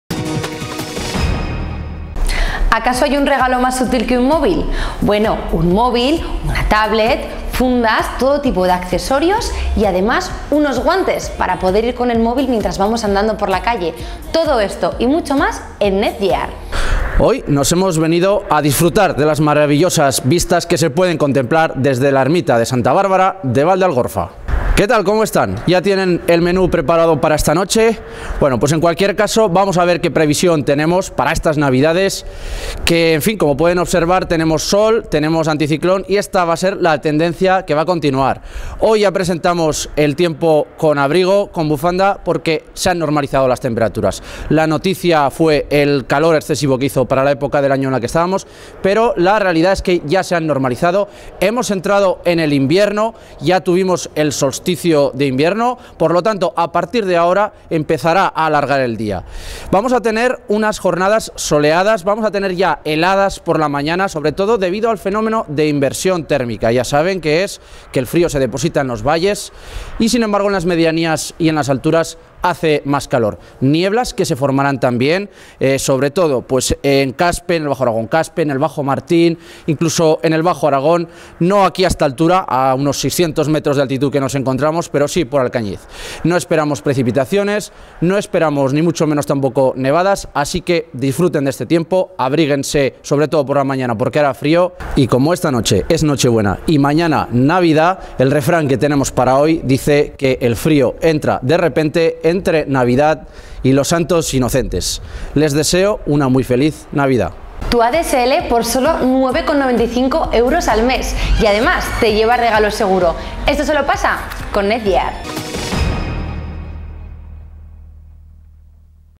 Pronóstico del tiempo para Navidad
Javier de Luna nos lleva hasta la ermita de Santa Bárbara en Valdealgorfa para informarnos del pronóstico del tiempo durante el puente de Navidad.